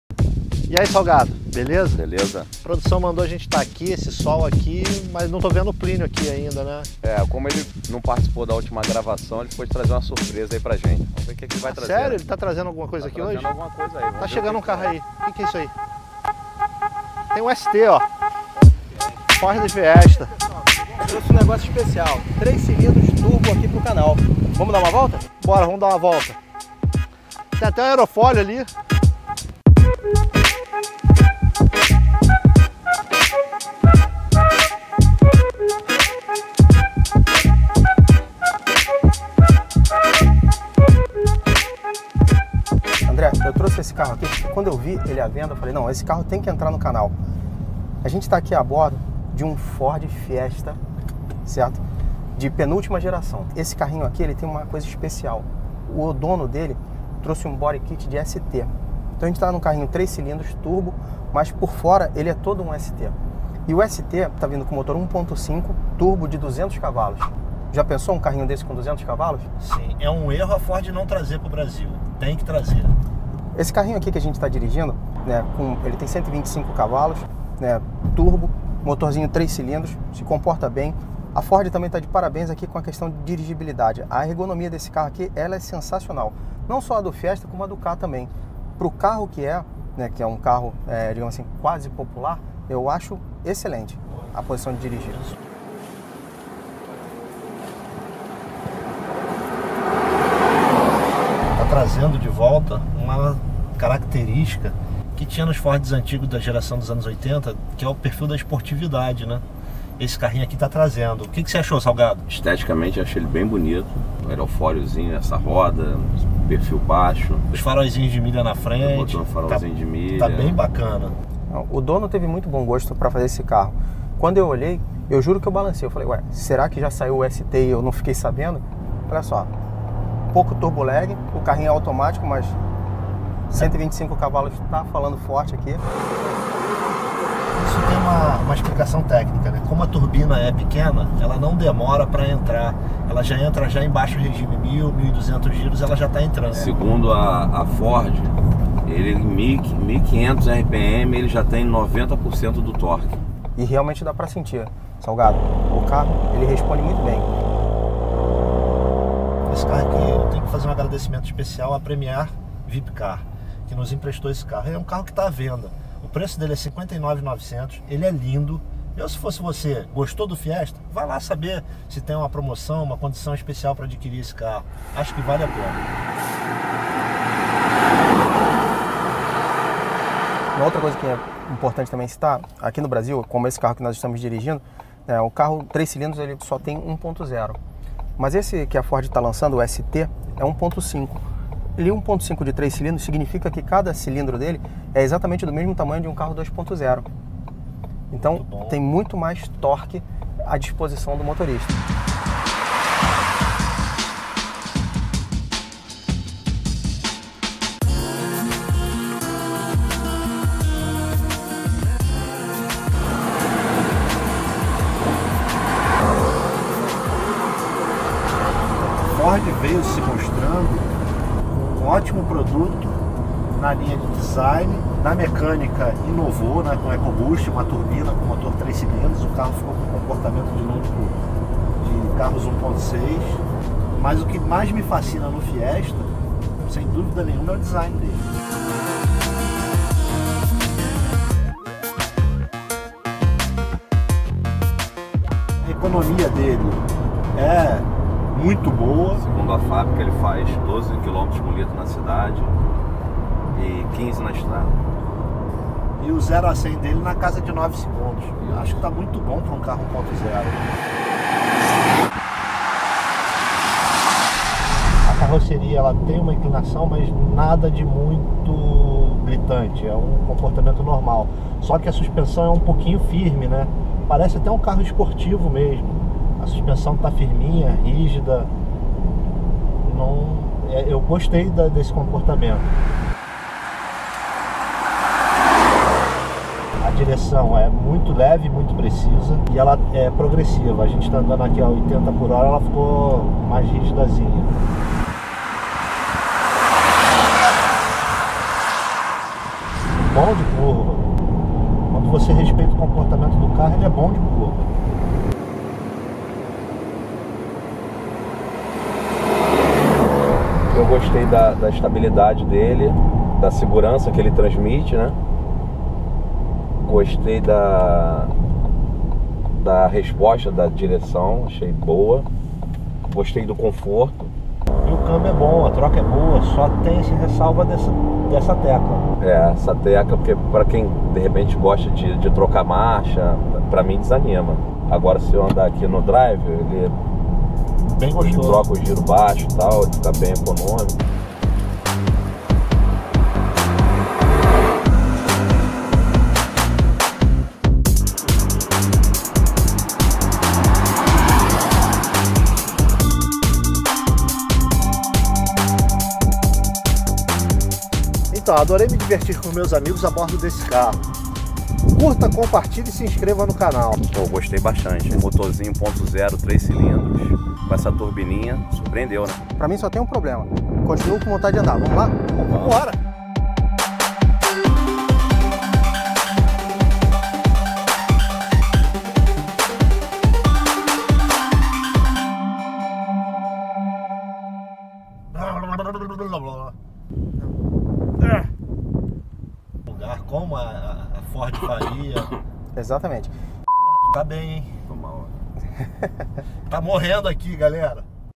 FORD FIESTA TITANIUM COM KIT ST - TESTE COMPLETO - SOLER REVIEW - EP70
Ford Fiesta 1.0 Titanium, com motor turbo de 3 cilindros mostrou-se um carro excelente e com um conceito muito legal para o uso urbano.
Ainda Ganhando um kit estético, com Spoilers, Para-choques e Faixas de Rally  e com novo conjunto óptico onde foi incorporado os faróis de milha Lucas seguindo ao padrão dos carros de Rally.
Desta forma o Fiesta tornou-se um carro bastante Jovial e descolado espero que gostem!

O nosso objetivo é criar diversão de maneira interativa ao público em geral.

Siga o Canal Soler no Facebook, YouTube e Instagram!